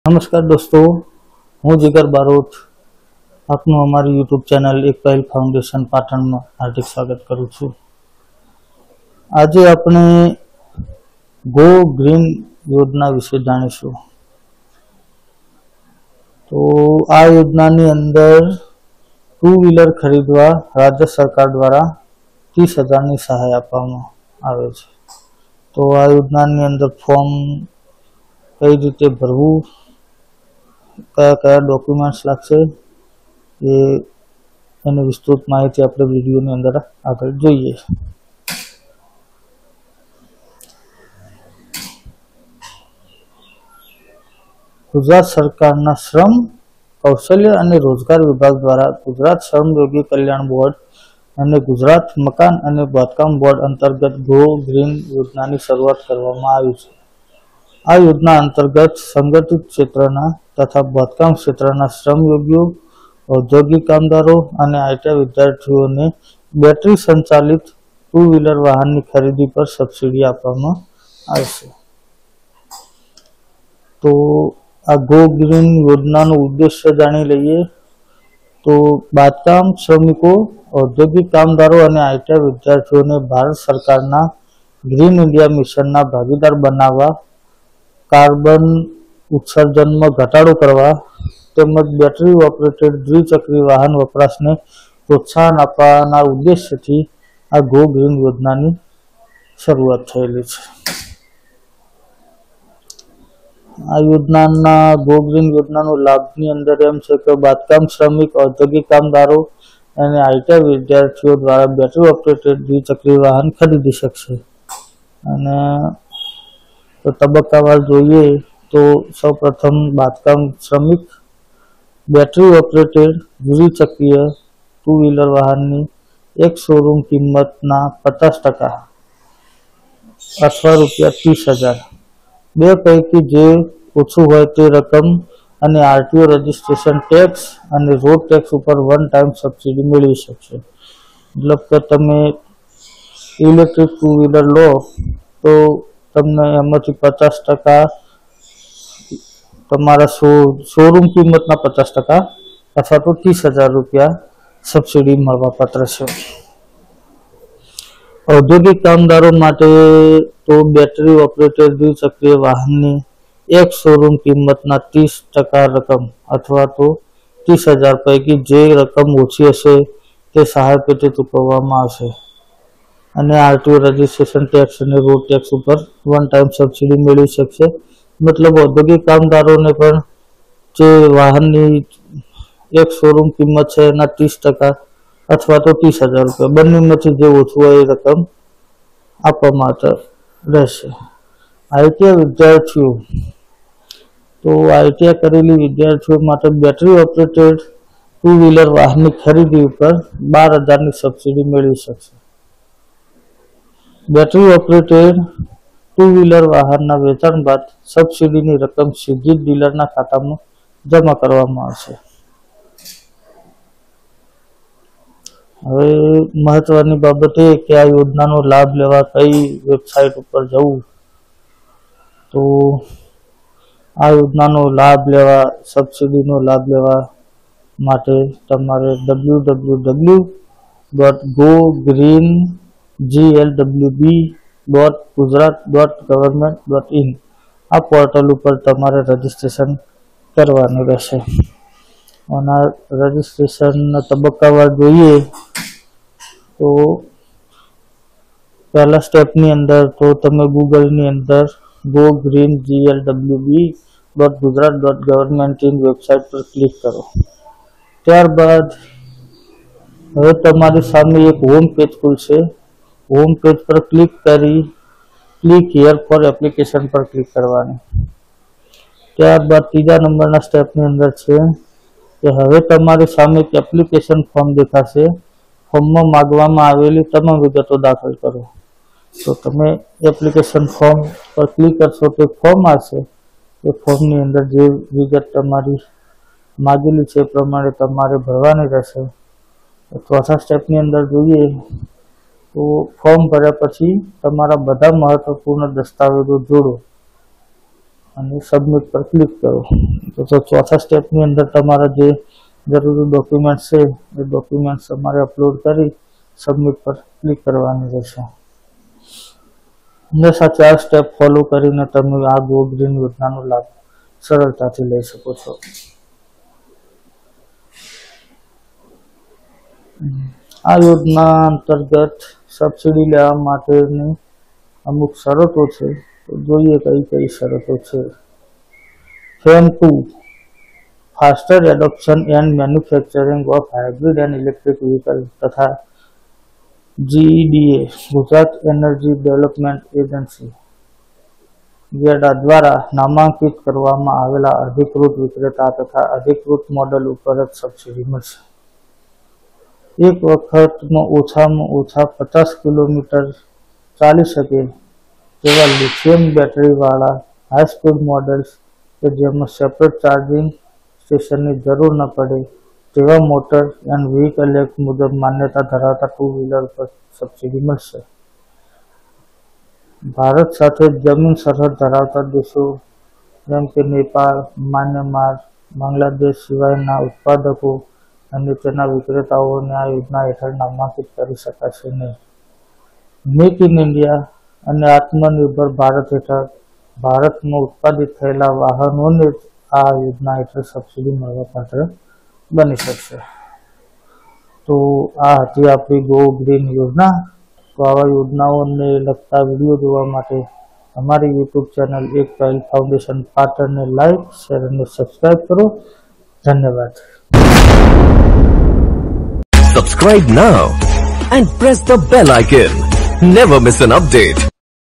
GO-GREEN યોજના-2023 l ટુ-વ્હિલર વાહનની ખરીદી પર 30% સબસિડી
આ યોજના અંતર્ગત સંગઠીત ક્ષેત્રના તથા બાંધકામ ક્ષેત્રના શ્રમયોગીઓ, ઔદ્યોગિક કામદારો અને ITI વિદ્યાર્થીઓને બેટરી સંચાલિત ટુ-વ્હિલર વાહનની ખરીદી પર સબસિડી આપવામાં આવશે. આ યોજના હેઠળ પ્રાપ્ત થતી સબસિડી મેળવવા માટે શ્રમયોગી ઑનલાઈન અરજી કરી શકે છે. બેટરી ઓપરેટડ દ્રી-ચક્રી વાહનના વેચાણ બાદ સબસિડીની રકમ સીધી જ ડીલરના ખાતામાં જમા કરવામાં આવશે.

ગુજરાત સરકારના શ્રમ, કૌશલ્ય અને રોજગાર વિભાગ દ્વારા ગુજરાત શ્રમયોગી કલ્યાણ બોર્ડ અને ગુજરાત મકાન અને બાંધકામ બોર્ડ અંતર્ગત GO-GREEN યોજનાની શરૂઆત કરવામાં આવી છે. આ યોજના અંતર્ગત સંગઠીત ક્ષેત્રના તથા બાંધકામ ક્ષેત્રના શ્રમયોગીઓ, ઔદ્યોગિક કામદારો અને ITI વિદ્યાર્થીઓને બેટરી સંચાલિત ટુ-વ્હિલર વાહનની ખરીદી પર સબસિડી આપવામાં આવશે.

GO-GREEN યોજનાનો ઉદ્દેશઃ
- બાંધકામ શ્રમિકો, ઔદ્યોગિક કામદારો અને ITI વિદ્યાર્થીઓને ભારત સરકારના "Green India" મિશનના ભાગીદાર બનાવવા
- કાર્બન ઉત્સર્જનમાં ઘટાડો કરવો
- બેટરી ઓપરેટેડ દ્વી-ચક્રી વાહન વપરાશને પ્રોત્સાહન આપવું

- બેટરી ઓપરેટેડ દ્વી-ચક્રી વાહન ખરીદી માટે સબસિડી પૂરી પાડવી

યોજનાનું નામ GO-GREEN યોજના
યોજનાનો ઉદ્દેશ બેટરી ઓપરેટેડ દ્રી-ચક્રી વાહનોના વપરાશને ઉત્તેજન આપવું
યોજનાના લાભાર્થીઓ બાંધકામ ક્ષેત્રના શ્રમિકો, ઔદ્યોગિક ક્ષેત્રના કામદારો અને ITIના વિદ્યાર્થીઓ
યોજના હેઠળ મળવાપાત્ર સબસિડી બાંધકામ શ્રમિકઃ 50% અથવા રૂ.30,000/-

GO-GREEN યોજનાના લાભઃ
- બાંધકામ શ્રમિક, ઔદ્યોગિક કામદારો અને ITI વિદ્યાર્થીઓ દ્વારા બેટરી ઓપરેટેડ દ્વિ-ચક્રી વાહન ખરીદી શકશે
- બાંધકામ શ્રમિકઃ બેટરી ઓપરેટડ દ્રી-ચક્રી વાહનની એક્સ-શો રૂમ કિંમતના 50% અથવા રૂ.30,000/- બે પૈકી જે ઓછી હોય તે રકમ અને RTO રજિસ્ટ્રેશન ટેક્સ અને રોડ ટેક્સ ઉપર વન-ટાઇમ સબસિડી મેળવી શકશે.
-ઔદ્યોગિક કામદારોઃ બેટરી ઓપરેટડ દ્રી-ચક્રી વાહનની એક્સ-શો રૂમ કિંમતના 30% રકમ અથવા રૂ.30,000/- પૈકી જે રકમ ઓછી હશે તે સહાય પેટે ચૂકવવામાં આવશે અને RTO રજિસ્ટ્રેશન ટેક્સ અને રોડ ટેક્સ ઉપર વન-ટાઇમ સબસિડી મેળવી શકશે.
- ITI વિદ્યાર્થીઓઃ બેટરી ઓપરેટડ દ્રી-ચક્રી વાહનની ખરીદી ઉપર રૂ.12,000ની સબસિડી મેળવી શકશે.
- બેટરી ઓપરેટડ દ્રી-ચક્રી વાહનના વેચાણ બાદ સબસિડીની રકમ સીધી જ ડીલરના ખાતામાં જમા કરવામાં આવશે.

આ વેબ પોર્ટલ પર કરી શકાશે અરજી
- આ યોજના હેઠળ પ્રાપ્ત થતી સબસિડી મેળવવા માટે શ્રમયોગી ઑનલાઈન અરજી કરી શકે છે.
- તે માટે પોર્ટલ ઉપર અરજી કરવાની રહેશે.
ગુજરાત ટુ વ્હીલર સ્કીમ 2022 માટે અરજી કરવાની પ્રક્રિયા

આ યોજના હેઠળ અરજી કરવા માંગતા અરજદારોએ સરકાર દ્વારા શરૂ કરવામાં આવેલી સત્તાવાર વેબસાઇટ મુલાકાત લઇને નીચેના પગલાં અનુસરવાના રહેશેઃ

STEP 1 - અહીં આપેલી લિંક ક્લિક કરો.
STEP 2 - હવે તમારી સામે એક હોમ પેજ ખુલશે. હોમપેજ પર "Click Here for Application" પર ક્લિક કરો.
STEP 3 - હવે તમારી સામે એક એપ્લિકેશન ફોર્મ દેખાશે. ફોર્મમાં માંગવામાં આવેલી તમામ વિગતો દાખલ કરો.
STEP 4 - ફોર્મ ભર્યા પછી, તમારા બધા મહત્વપૂર્ણ દસ્તાવેજો જોડો હવે સબમિટ વિકલ્પ પર ક્લિક કરો
- આ પગલાં અનુસરીને તમે તમે ગો-ગ્રીન યોજના(Go Green Shramik Yojana Gujarat) હેઠળ સરળતાથી અરજી કરી શકો છો